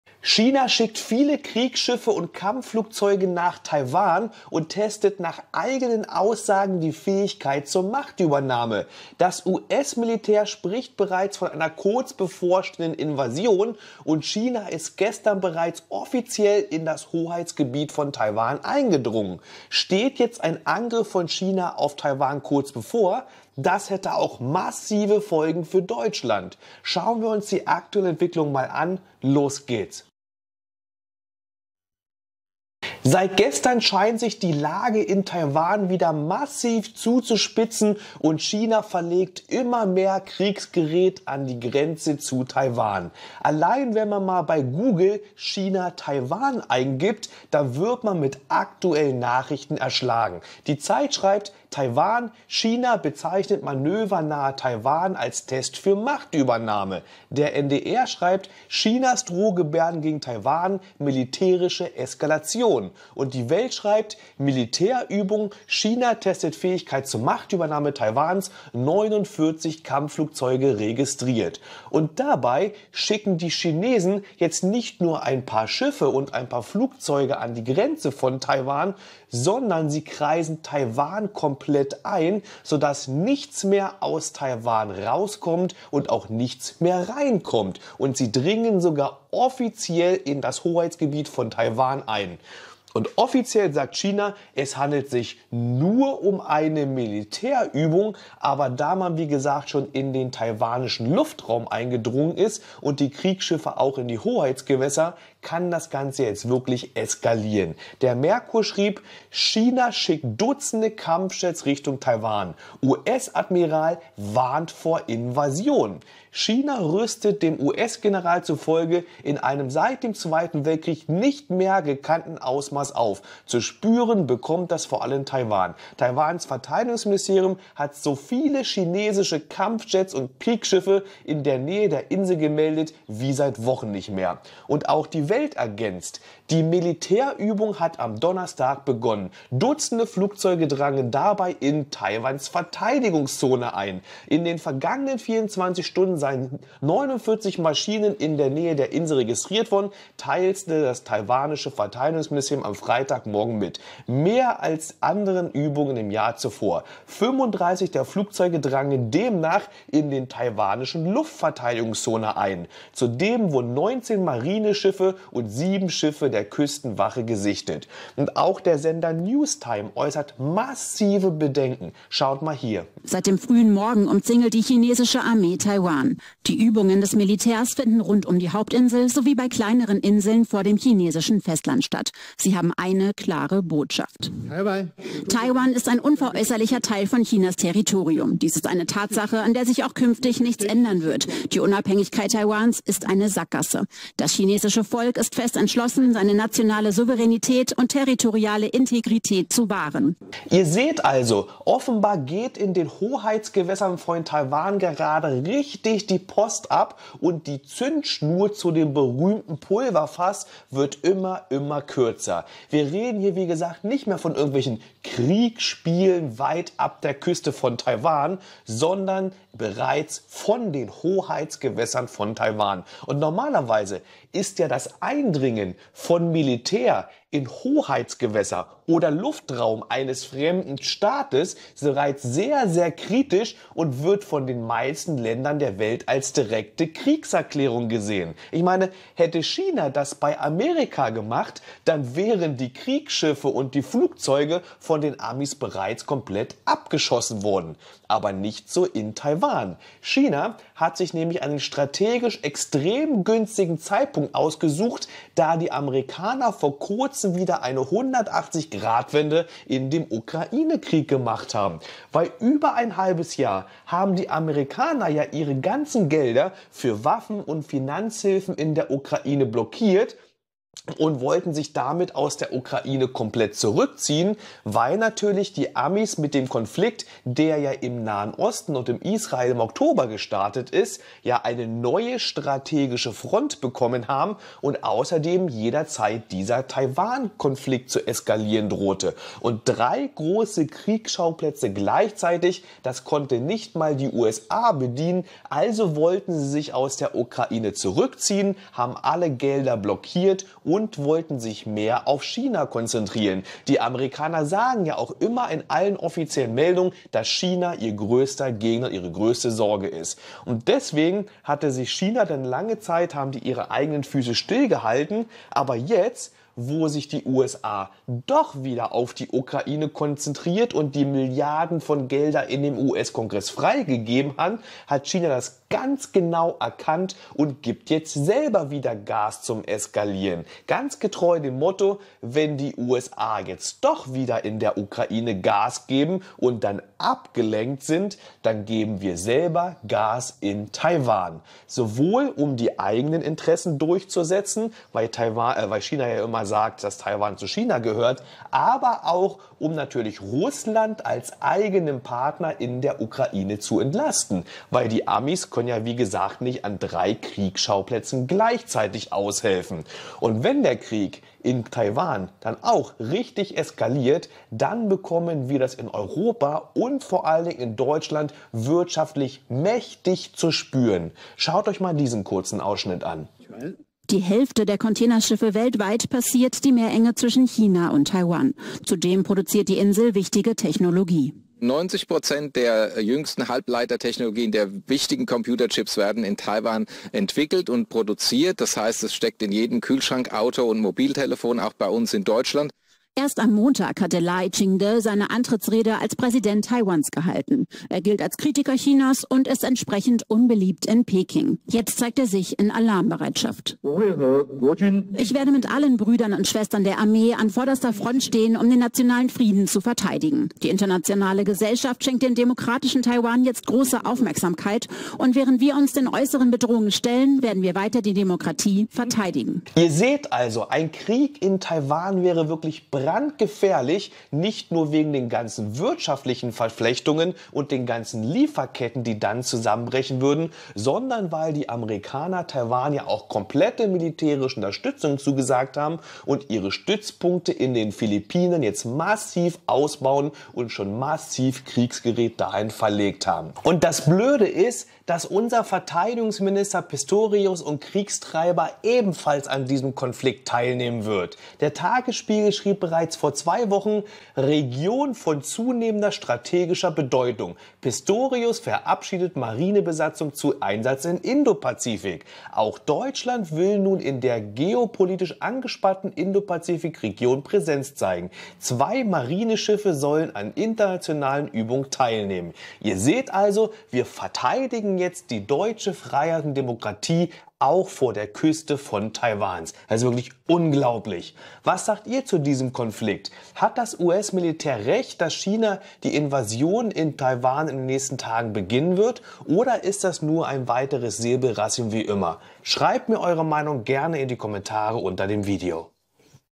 Chinesische Kriegsschiffe & Flugzeuge dringen in Taiwans Verteidigungszone ein!
China schickt Kriegsschiffe & Kampfflugzeuge nach Taiwan und testet nach eigenen Aussagen die Fähigkeit zur Machtübernahme. Das US-Militär spricht sogar von einer kurz bevorstehenden Invasion und China ist gestern schon in das offizielle Hoheitsgebiet von Taiwan eingedrungen. Steht der Angriff von China auf Taiwan kurz bevor? Das hätte auch massive Folgen für Deutschland.